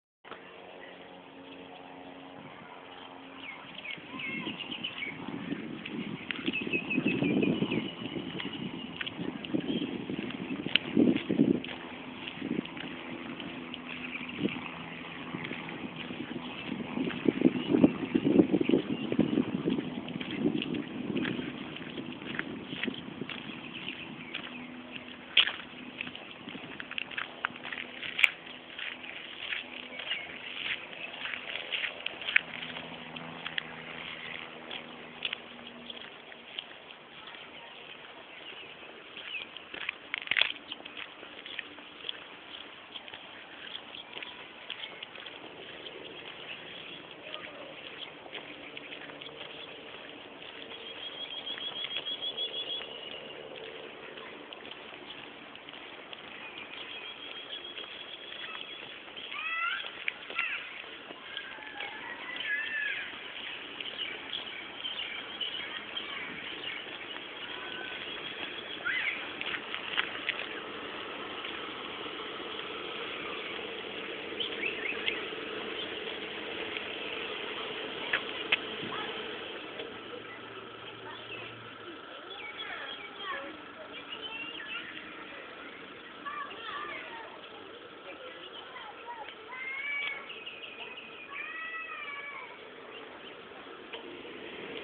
Tin Mining Dredge at Tanjung Tualang, Malaysia
The only surviving tin mining dredge in Perak, Malaysia. Kinda said as it was tin that built the Perak state (tin in Malay is Perak).